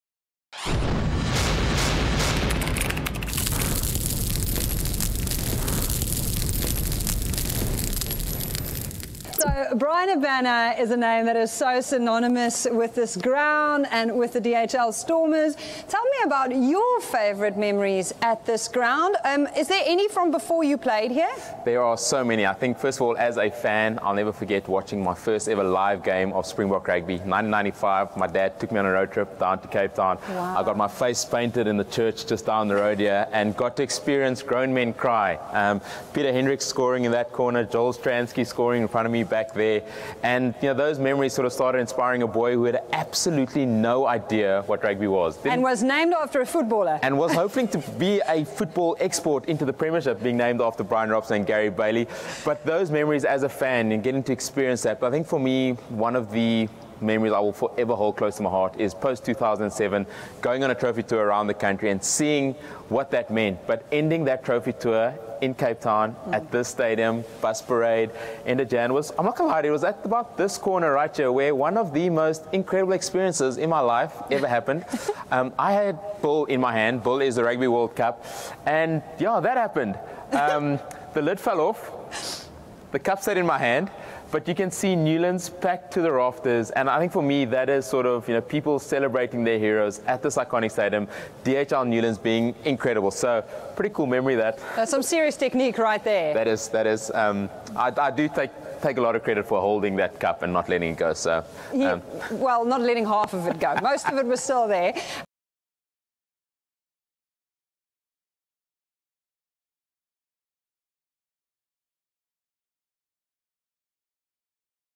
BrightRock Light a Fire for Newlands Bryan Habana
“It was at this stadium that one of THE most incredible experiences in my life ever happened”.
Bryan Habana tells Elma Smit about some of his own special memories of DHL Newlands. For Bryan, many special moments occurred not only as a player, but as a fan as well. He recalls driving down to Cape Town in 1995 with his dad and getting to “experience grown men cry”, which became an inspirational moment for him. Watch to hear him share his ultimate DHL Newlands memory, and also find out who “Bill” is.See the full show